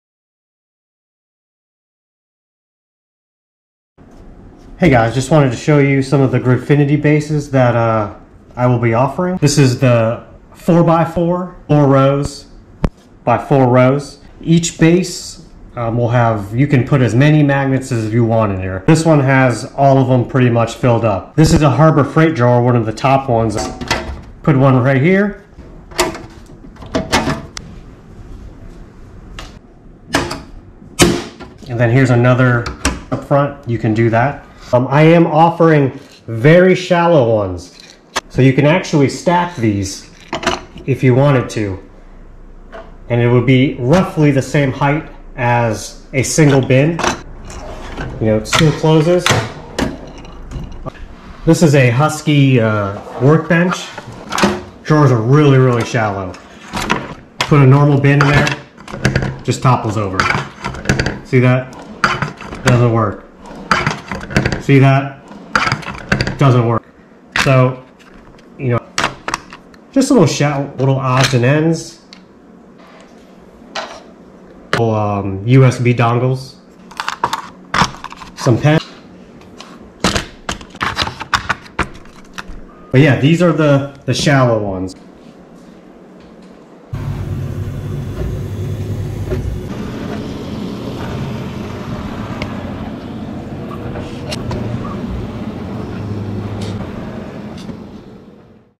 Gridfinity Overview | Modular Organization
Gridfinity bins and baseplates now available at draildiagnostics. Thanks to the blessing of Zach Freedman the original creator I now have them available for sale.